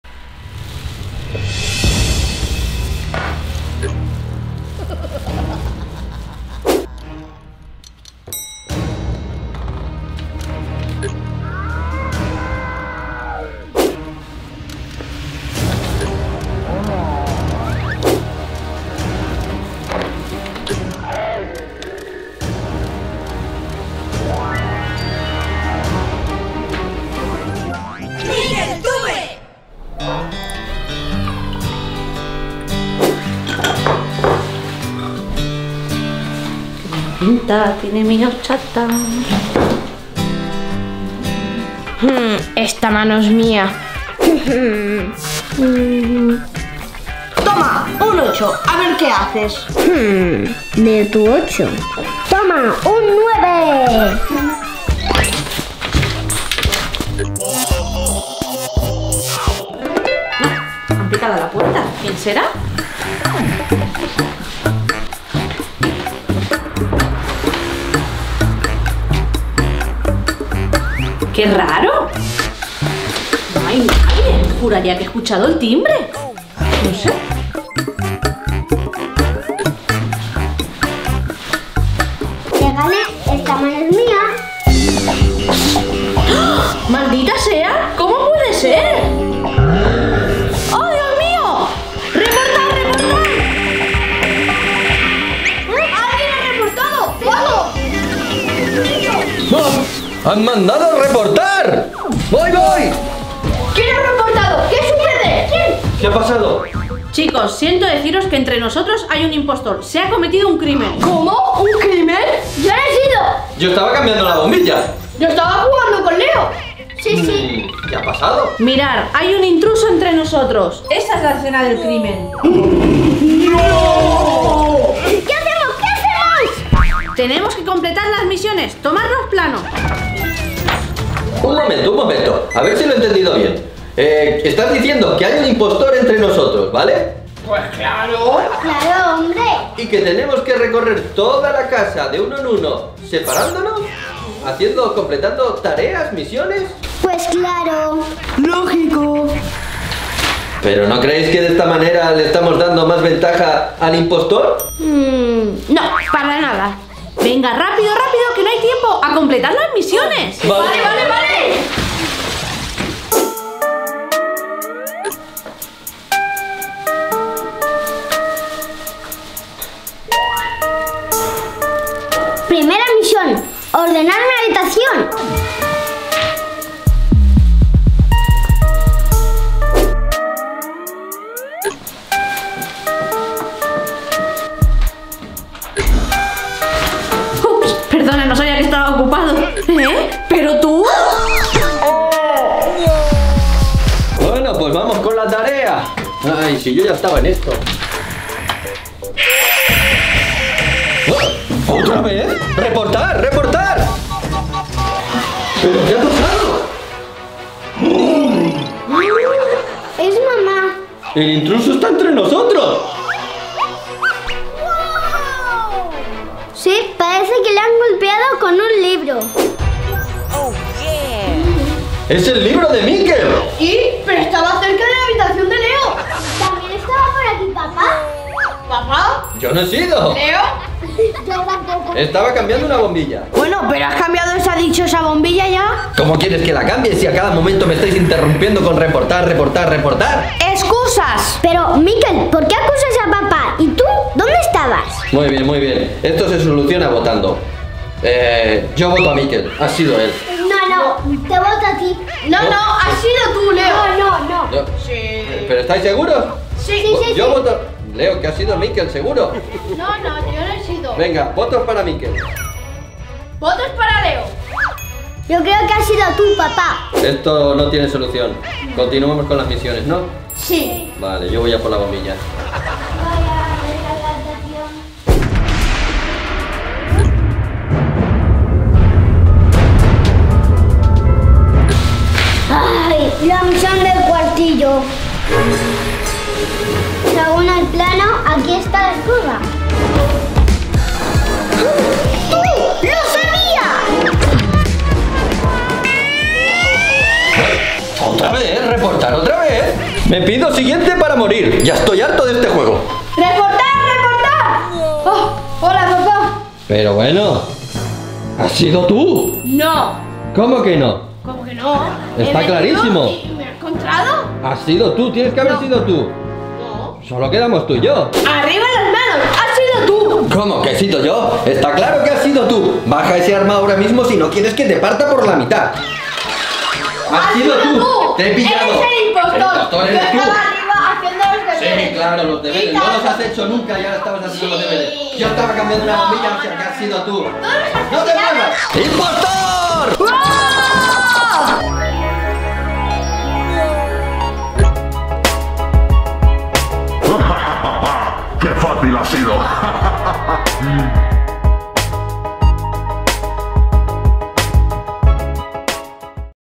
Among Us en la Vida Real
Hay un Impostor entre nosotros !! Quien será ? Among Us en la vida real.
Inspirados en el popular y divertido Videojuego del momento.
Este canal esta grabado y supervisado por adultos.